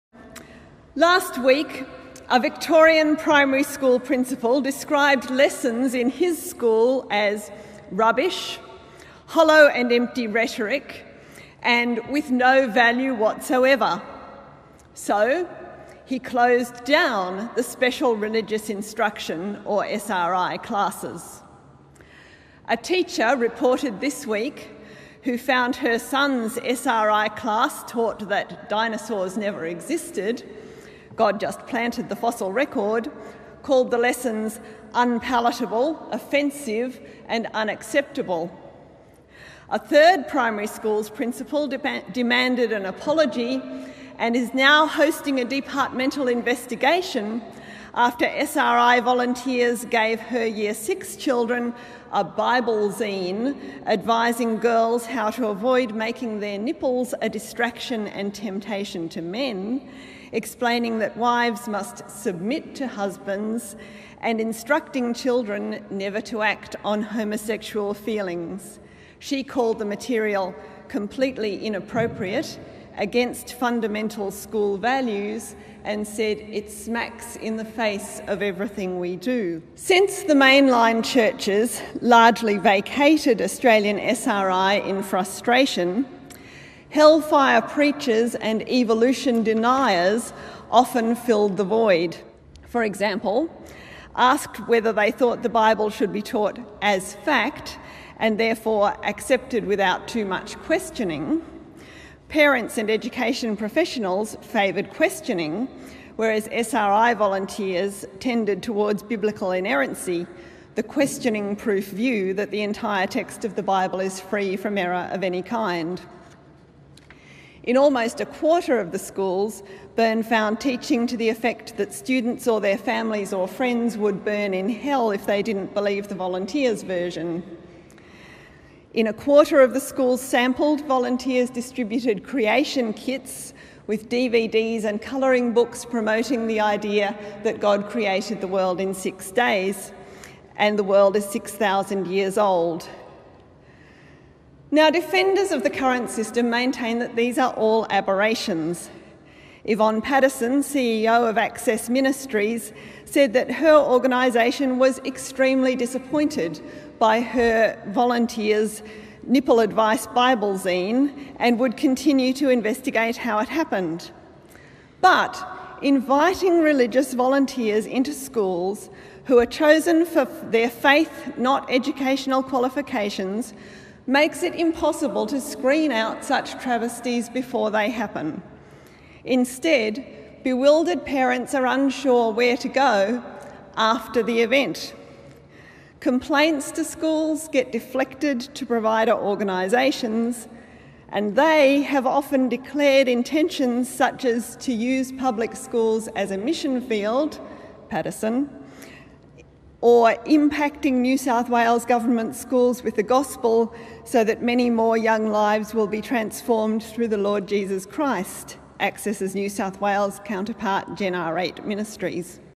Marion Maddox: IQ Squared Debate - R.E. in schools
"Faith-based Religious Education Has No Place in Public Schools"
Marion Maddox speaking for the affirmative.
DEBATE SUMMARY:
By the late 19th century, all Australian colonies had decreed that education must be compulsory, free and secular. By the middle of the twentieth century, 'secular' had gone out of fashion, seeing special religious instruction in all states.
Today most primary schools in Australia must allow for special religious instruction. In this incendiary first debate of 2014, our speakers will examine what it means to move away from the secular model and towards a spiritual one; debating the merits of our current model of volunteer church run instruction, as opposed to a balanced study of comparative religions, and contesting where such religious instruction sits within the central tenet of Australian democracy -- the separation of church and state.
Participants will include Professor Marion Maddox, author of Taking God to School: The End of Australia's egalitarian education?, award-winning author David Vann, Baptist Minister and World Vision Australia CEO Tim Costello, Dr Justine Toh, author and columnist Nick Cater and Professor Peter Sherlock.